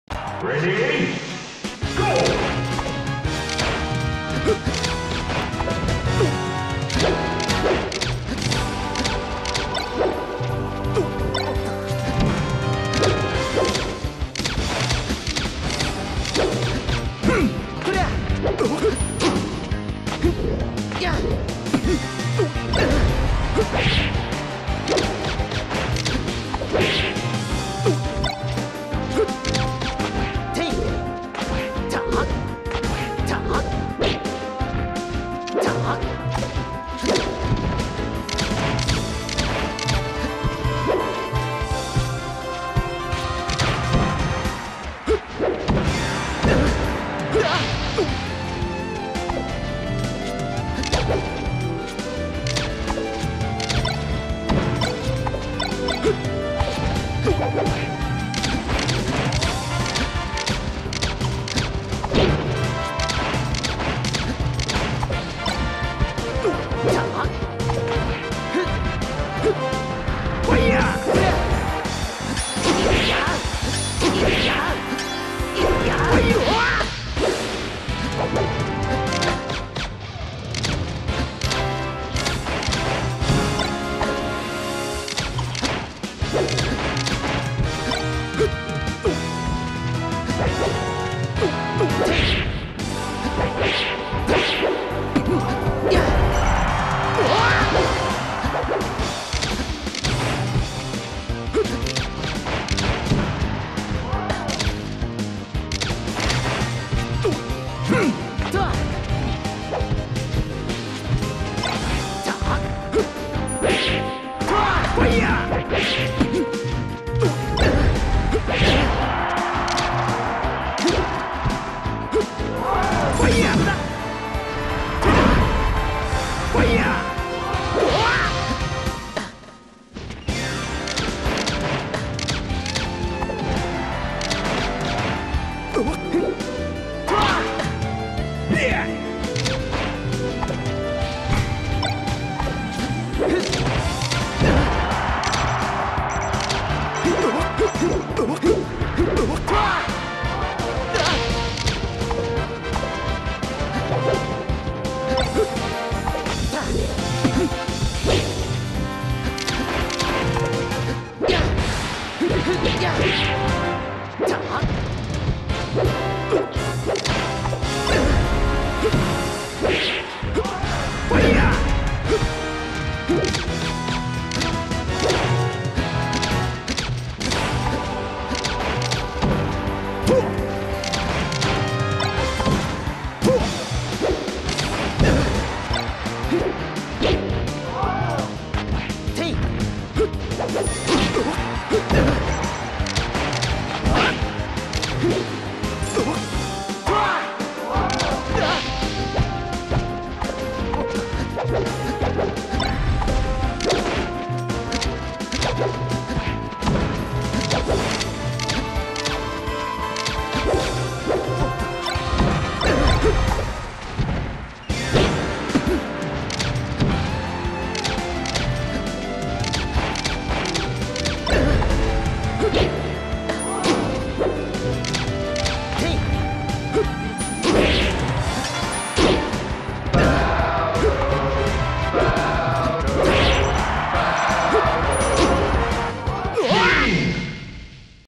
me14k (Falco) vs metroid1117 (Marth) 3 11_28_08.mpeg
11/28/08

Wow, I suck at chainthrowing.